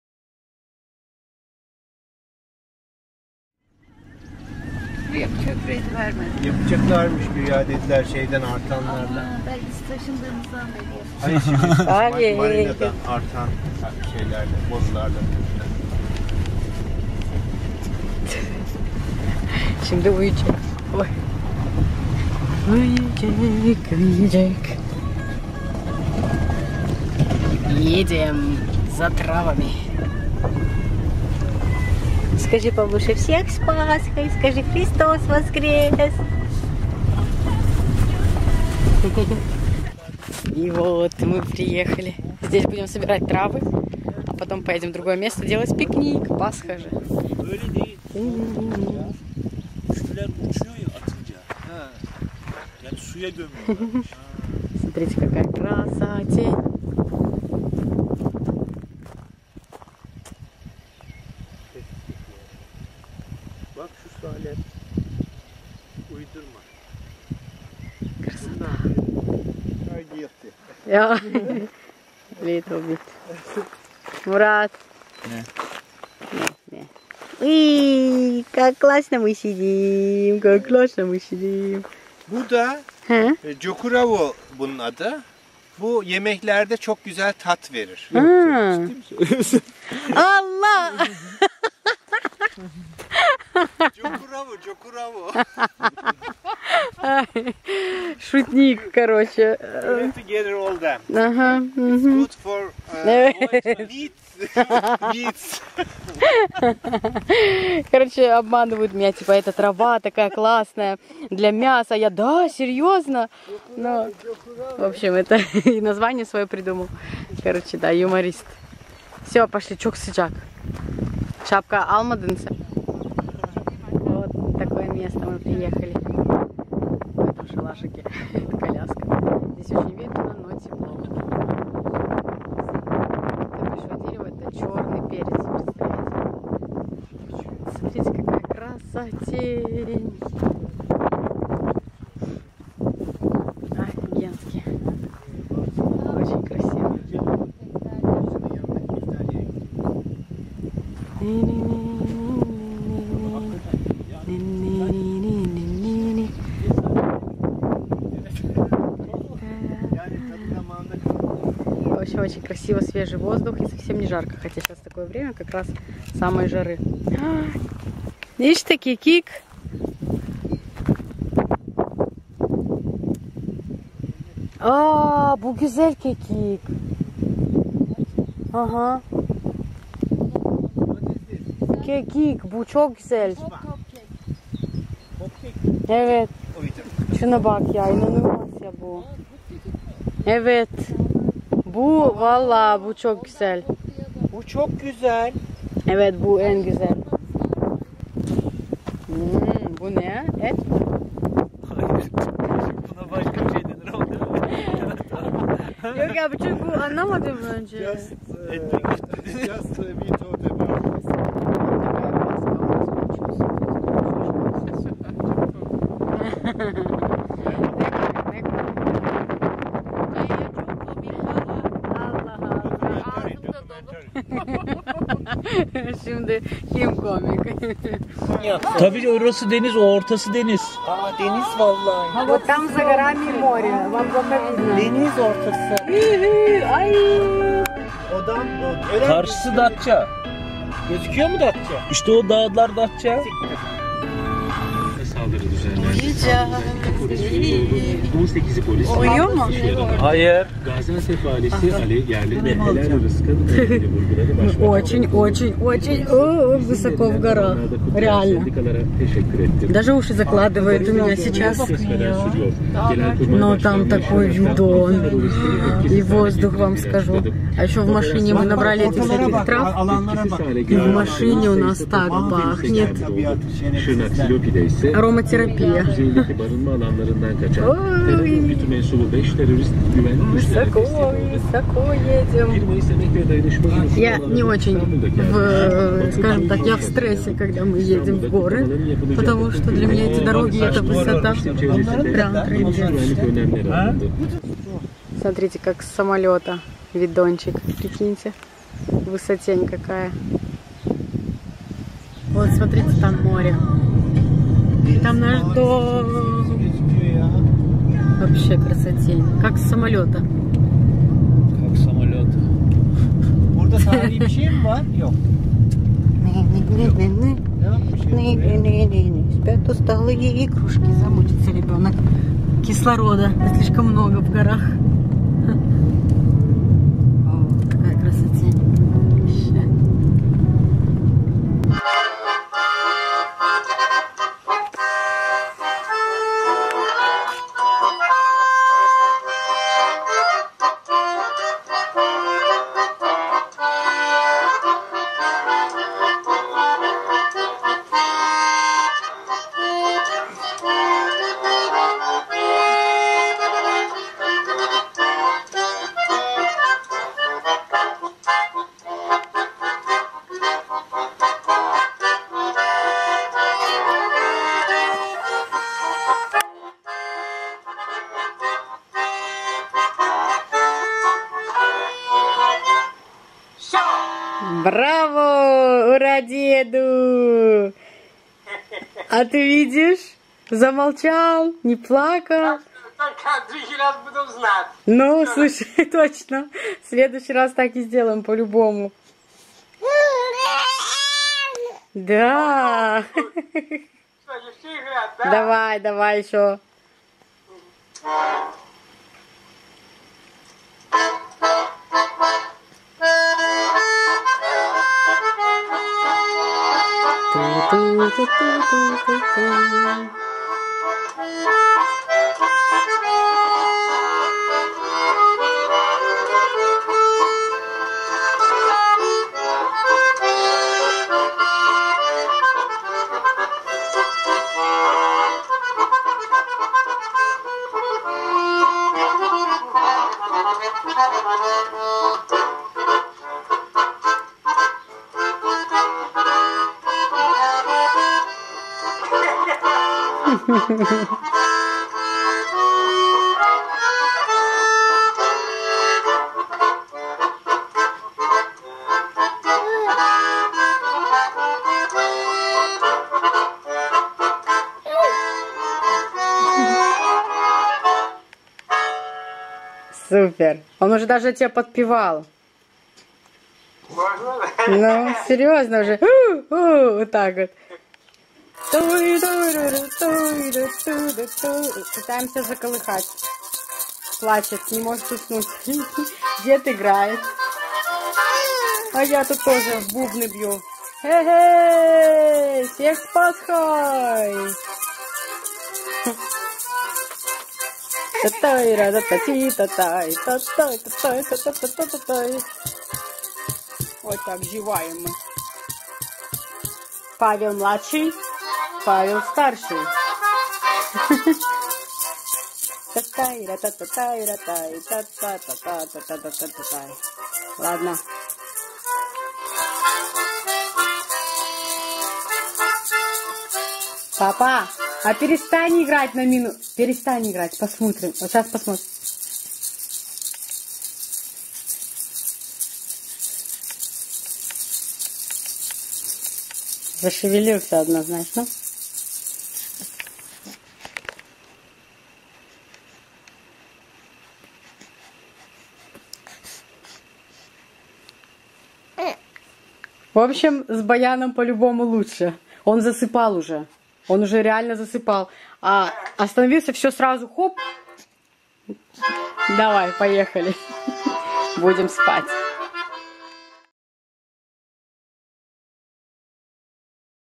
Первый день Пасхи Моя семья Жизнь в Турции Друзья Пикник
В этом видео я покажу, как мы провели первый день Пасхи в Турции с моей семьей и друзьями.